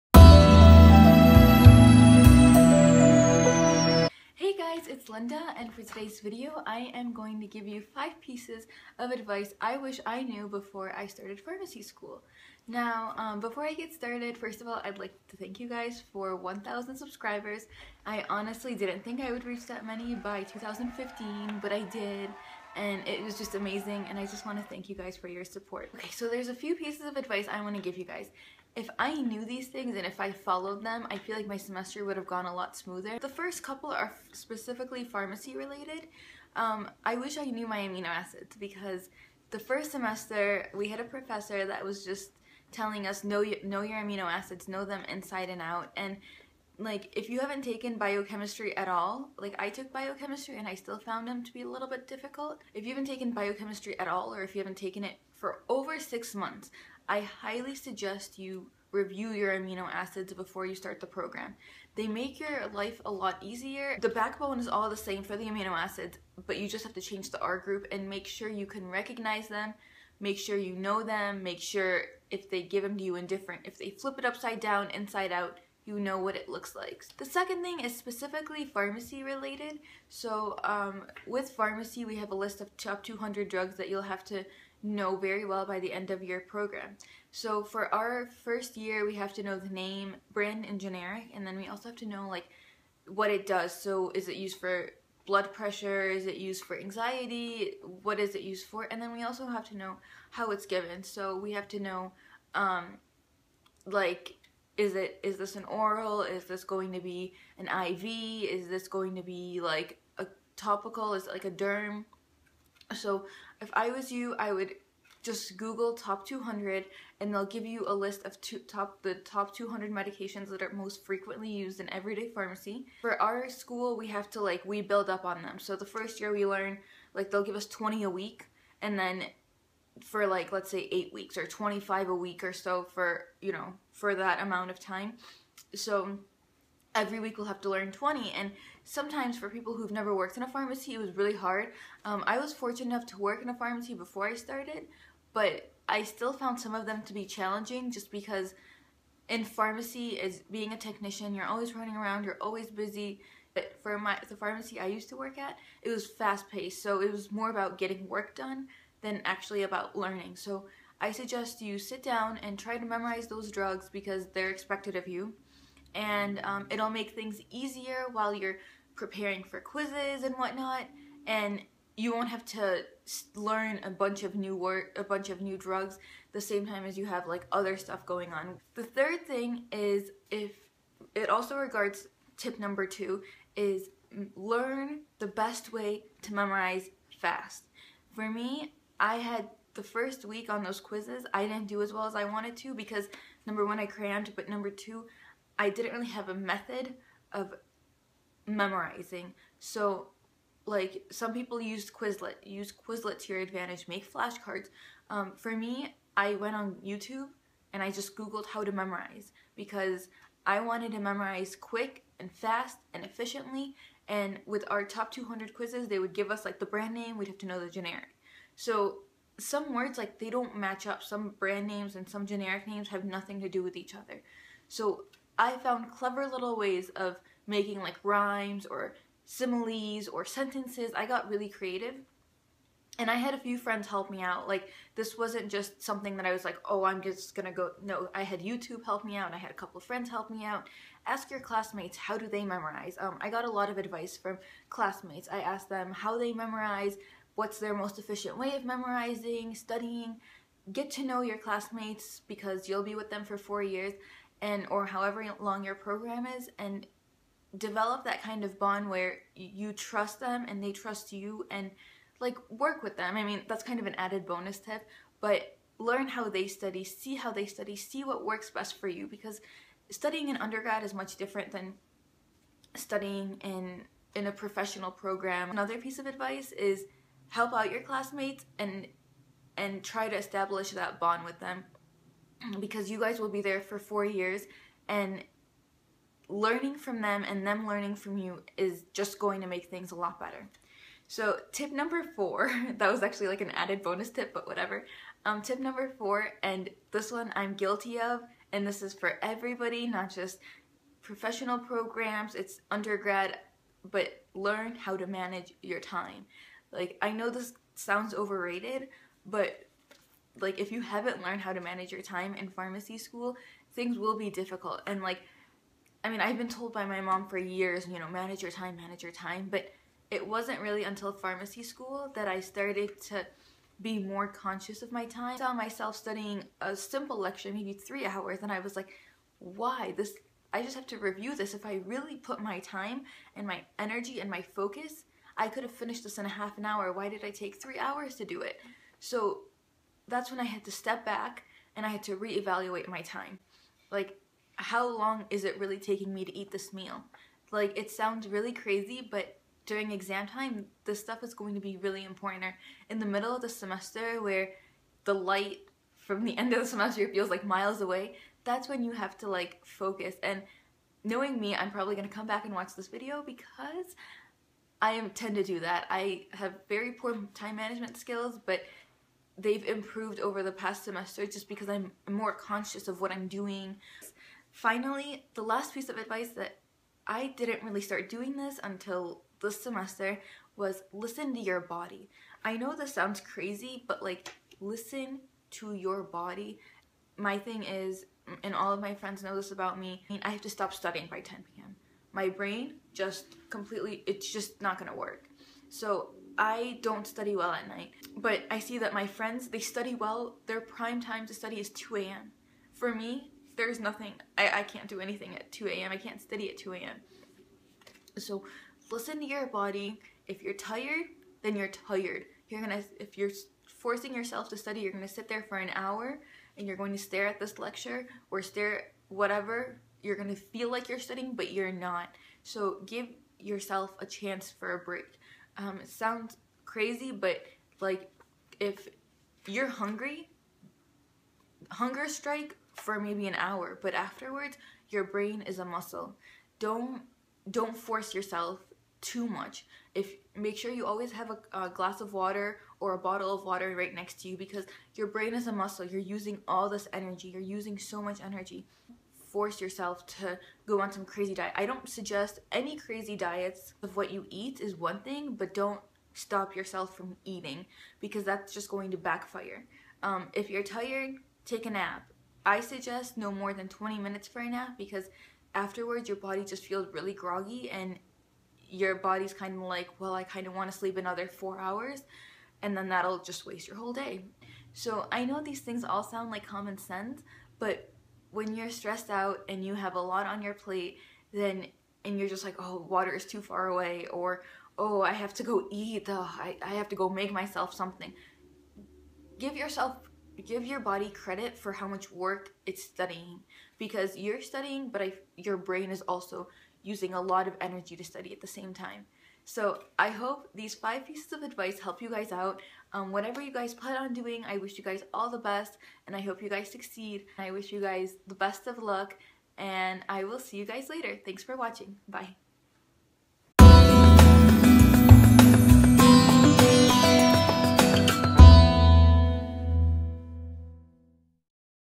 5 Things I Learned My First Year of Pharmacy School
Advice that will help you survive your first year of Pharmacy School!